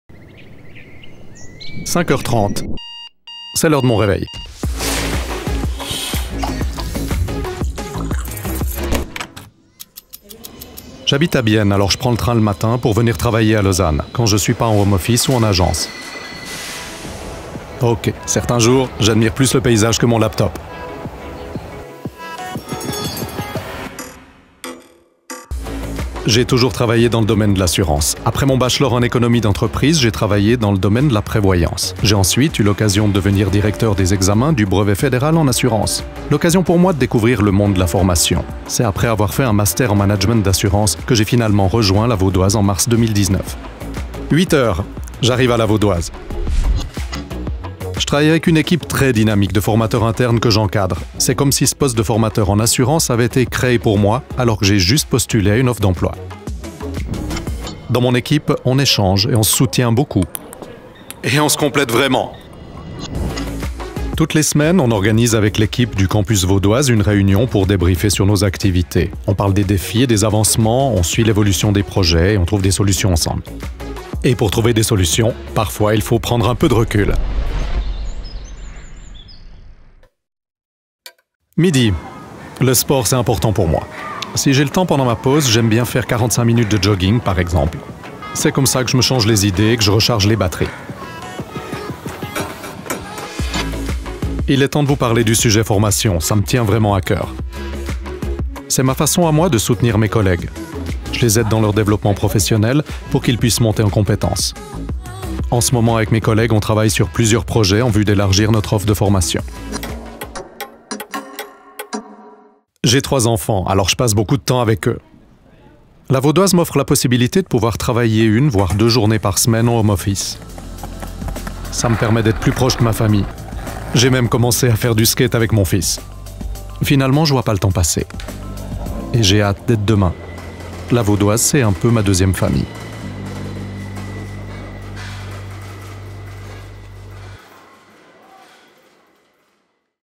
Profil Stefan
Nous offrons un environnement de travail sain et stimulant proposant de nombreuses opportunités professionnelles dans des métiers variés.
Rencontrez Stefan du Siège qui partage son expérience.